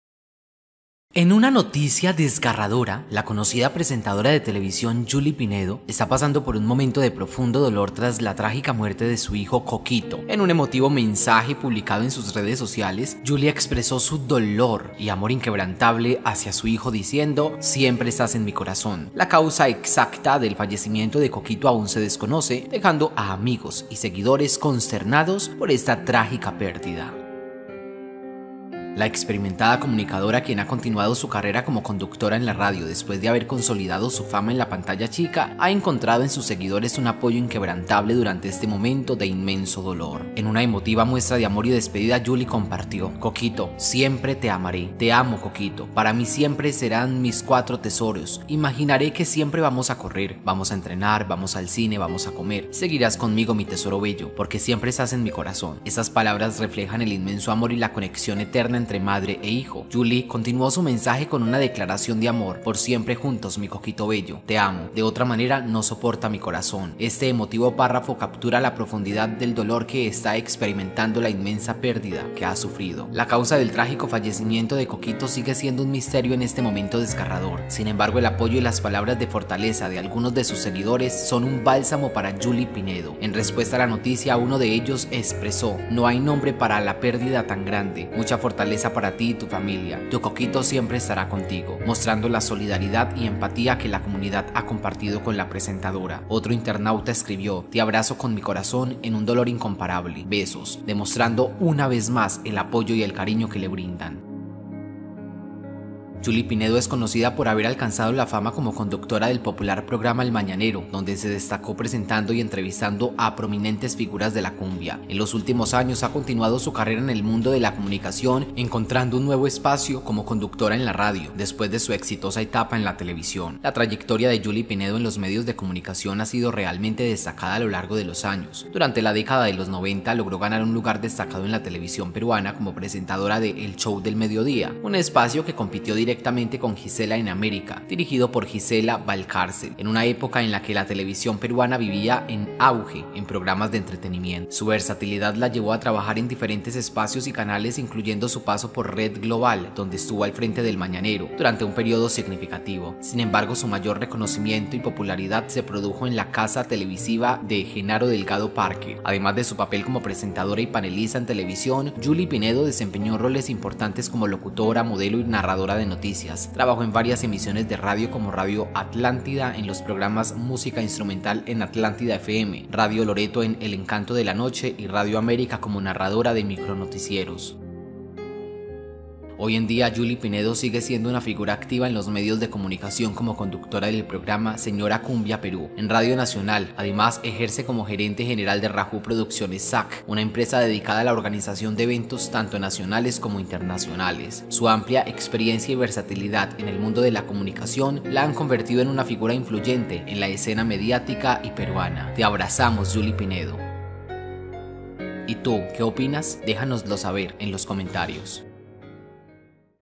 FALLECIÓ HIJO de JULY PINEDO en EXTRAÑAS CONDICIONES | PERÚ ESTÁ DE LUTO
"Coquito", el hijo de la recordada presentadora July Pinedo, falleció en extrañas condiciones.